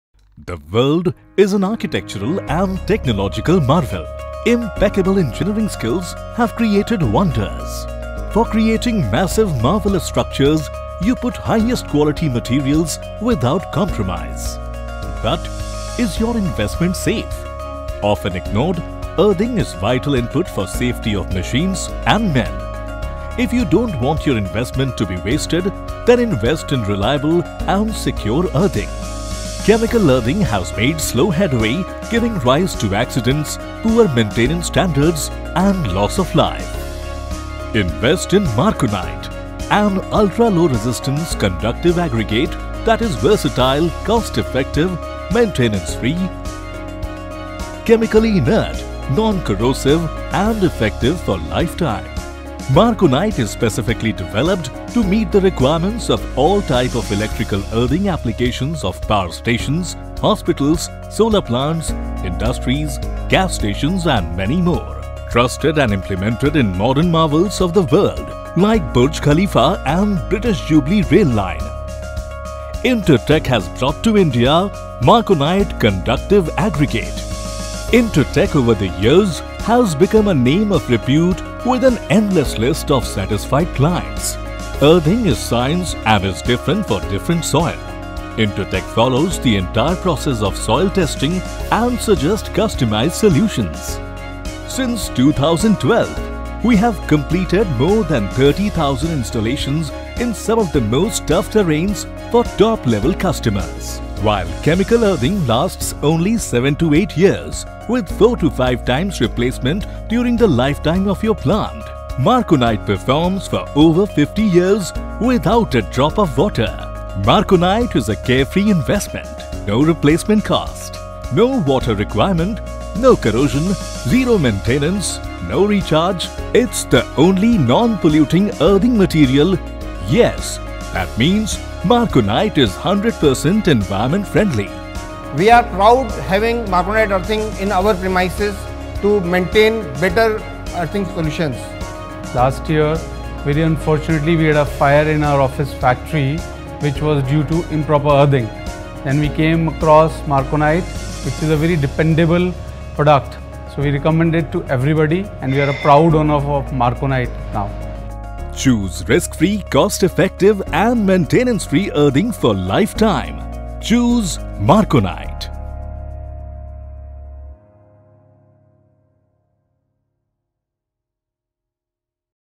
INTER-TECH Corporate Video
4th Generation Earthing (Marconite)

Charanjeet Singh
Founder & Partner Inter-Tech - Best Earthing Solution

Marconite® by Intertech is a conductive aggregate that provides enduring, stable as well as low resistance Earthing solutions that are effective in several complex ground conditions. It enables the electrical engineers to get a stable, permanent & low resistance earthing solution that helps the engineers handle the most complex soil conditions & get right earthing solutions across the world.

Marconite is manufactured in an extremely careful manner by the specialists and is packed in UV resistant valve topped poly sacks & palletized. Marconite earth electrodes have a better track record & are utilized for important Earthing solutions to a variety of soils.

This type of Earthing is dark grey & has a granular consistency. Marconite is the perfect alternative to chemical Earthing.

Marconite is a synthetic material manufactured specifically for use in earthing applications. Its distinctive properties result from a unique manufacturing process, utilising specific raw material feedstocks, carefully selected and mixed in tightly controlled ratio before undergoing a range manufacturing process and thermal treatments.

Marconite® is a dark grey, granular material that replaces traditional sand and aggregate materials used within concrete mixes. Marconite® is designed, developed and manufactured specifically for use in electrical earthing.

This results in a highly reliable earthing solution with the following unique benefits :

Low resistivity : 0.001Ωm is extremely low when compared to Bentonite’s 3Ωm.
Versatile : suitable for most ground conditions and becomes a permanent, solid structure that it is not prone to shrinking, drying out or being washed away.
Cost effective : it is a permanent solution; there is no need to remove and replace or ‘maintain’ it with additional water / salts every few years in order to achieve the desired earth values.
Chemically inert : is non-corrosive to steel or copper, does not attack cement structures and has a pH within the neutral range.
High strength : can be used as part of the building structure itself and can achieve strengths higher than Grade 25 concrete.
Easy to use : forms a concrete like material that from first pour, achieves a low resistance earth, no need to wait or return to test.

Marconite® is normally mixed with cement and water. However, it is also available in the form of a ‘premix’. This is a standard 25 kg poly sack which contains pre-weighed amounts of Marconite® and cement powder in the correct ratio to form the preferred concrete.

With the addition of 5 liters of water, each bag can then be mixed independently and used without the need to carry additional cement powder. Utilizing Marconite Premix reduces the possibility of incorrect mixing as work crews only carry one type of bag thus preventing on site confusion.

HISTORY OF MARCONITE
In the year 1972 PMC Carbon & Mr. Guglielmo Marconi developed Marconite & it was showcased to the whole world on “Tomorrow’s World”, a technology program of UK in the year 1977.

In 2002, Marconite’s patent right was bought by James Durrans & Sons Ltd, UK. Since then, they are manufacturing Marconite through their company named Carbon International Ltd. UK.

In 2011, Carbon International Ltd. UK appointed Intertech, based in New Delhi to be their prime Marconite distributor for India.